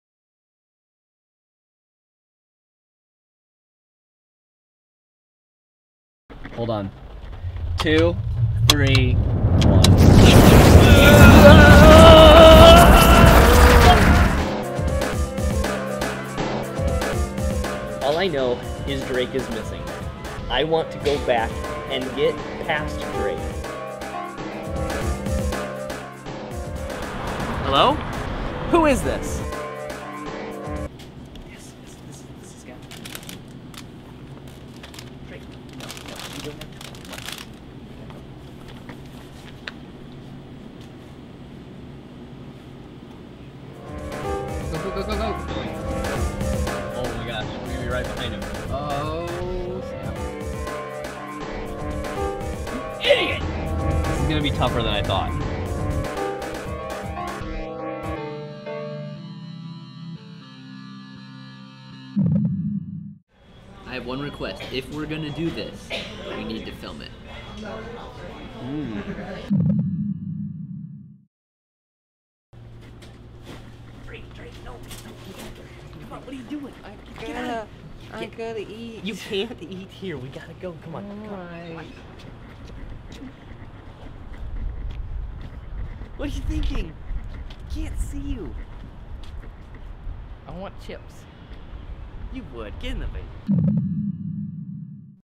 Buck and Drake III: Buck to the Future | Trailer 2
Here is the second theatrical trailer for Buck and Drake III: Buck to the Future. Drake has gone missing. It is up to Buck to travel back into the past and grab Drake to help in the search for Drake. The Gilleand Brothers star in and directed this film set to be released later this year.